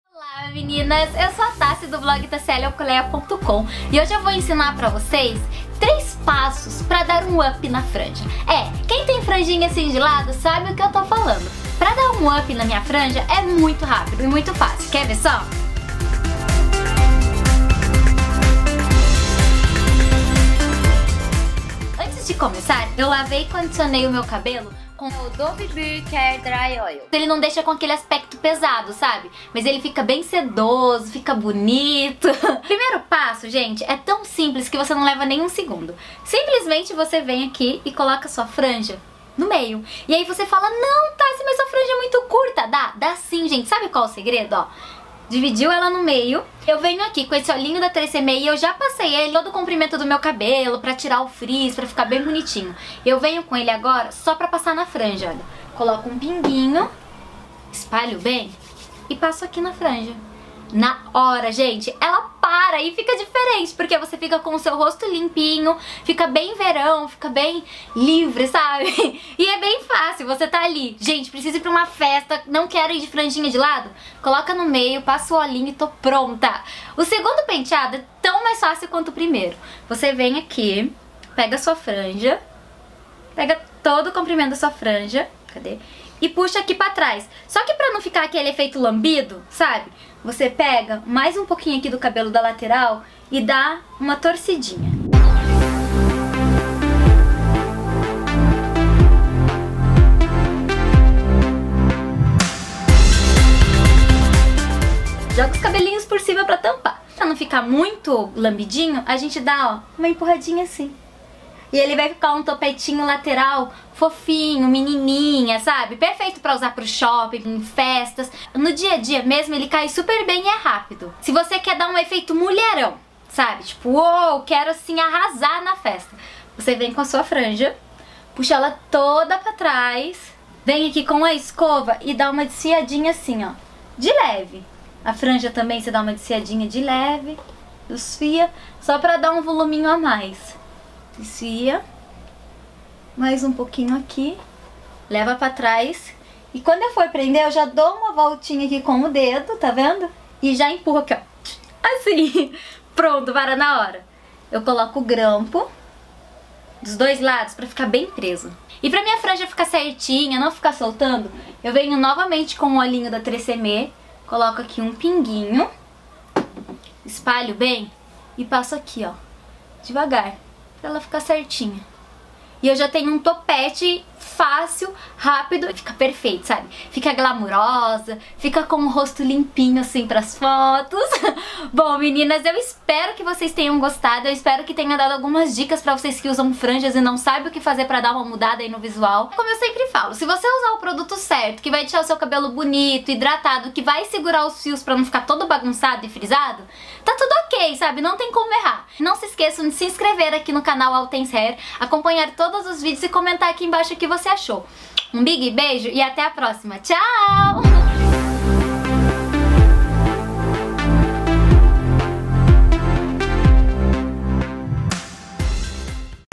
3 Penteados com Franja por Taciele Alcolea | All Things Hair™
Neste vídeo, Taciele Alcolea te ensina 3 penteados simples com franja. Ideal para quem quer variar o visual e conquistar resultados diferentes modelando e prendendo a franja.
Saiba mais sobre os produtos usados pela Taciele Alcolea:

➤ Shampoo e Condicionador Dove® Pure Care Dry Oil™ Advanced Hair Series

Todas as opiniões e conselhos deste vídeo são da própria Taciele. Visite o canal dela: